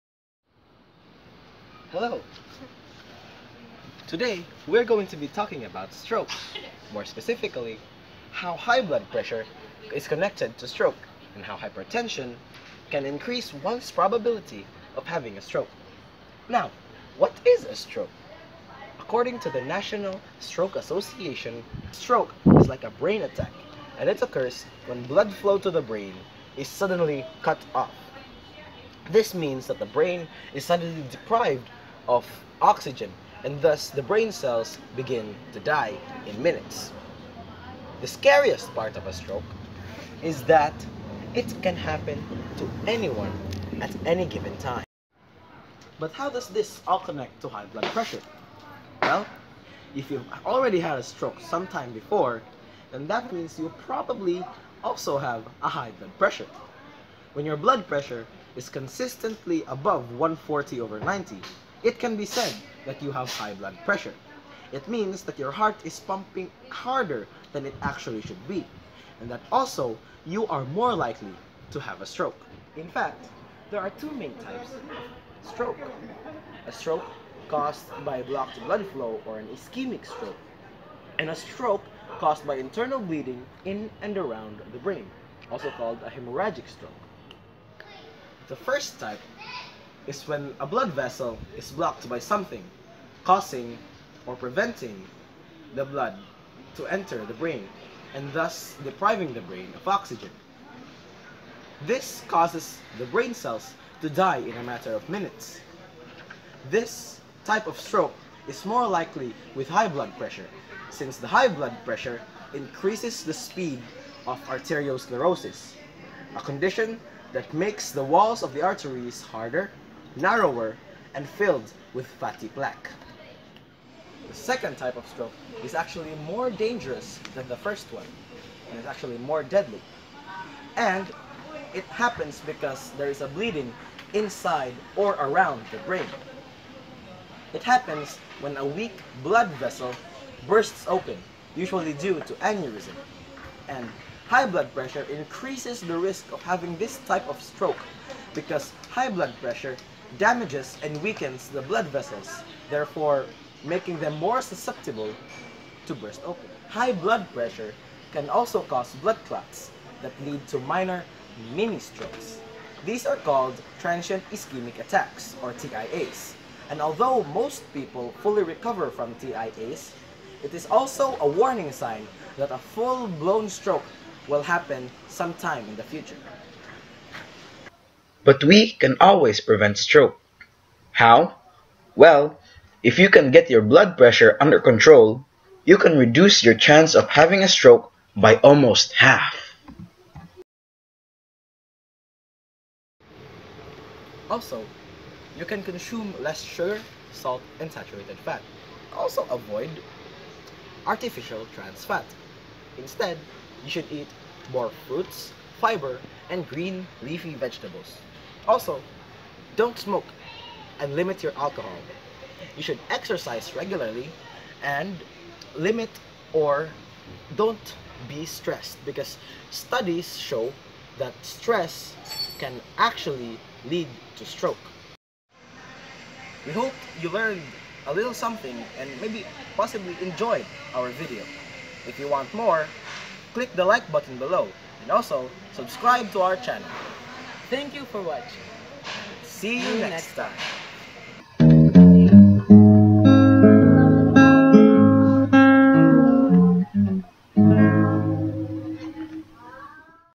STROKE and HIGH BLOOD PRESSURE
In this video, we will be talking about stroke, and how high blood pressure is connected to stroke.

This video was made by:

Nathaniel Ben Rosh Jovita
Seth Donal
Princess Ethel Atillo
Janice Carbos

Edited using VideoPad by NCH Software

Special thanks:
Ms. Alexandra Angcon, camera-person
Cebu Conference Inc., for use of facilities